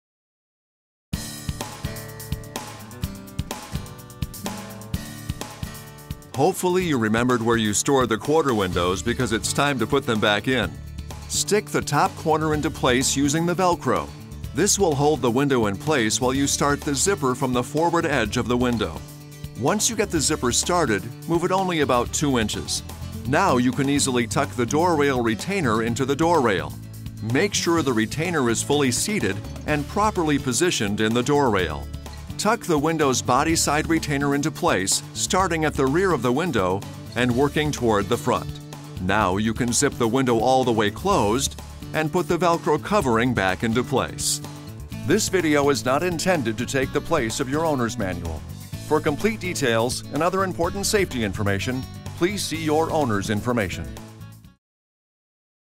Quarter Window Installation-Need windows replaced after convertible use on 2018 Jeep Wrangler (JK)
Your 2018 Jeep Wrangler features a cool, easy-to-use convertible top system. After you've enjoyed some open-air driving, you'll need the windows replaced. This video details the steps and procedures involved in Jeep window installation.